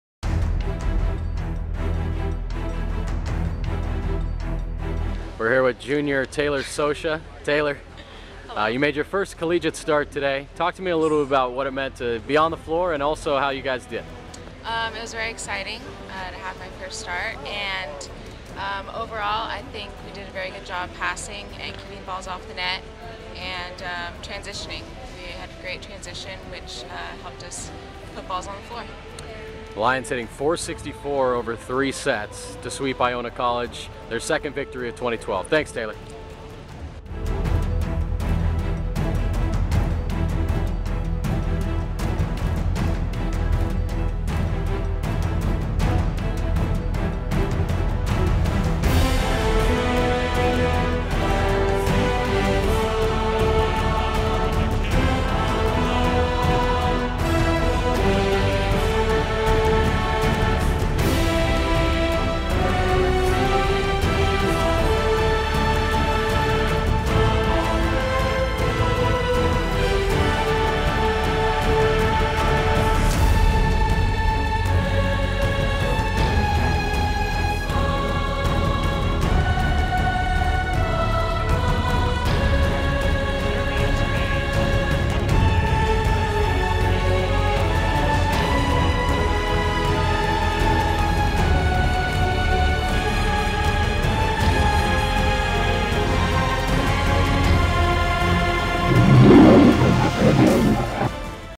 LMU Lions vs. Iona College Gaels Women's Volleyball Highlights
Highlights of the LMU Lions 3-0 victory over the Iona College Gaels in NCAA women's volleyball from the DoubleTree Los Angeles Westside Invitational tournament on Saturday, Aug. 25, 2012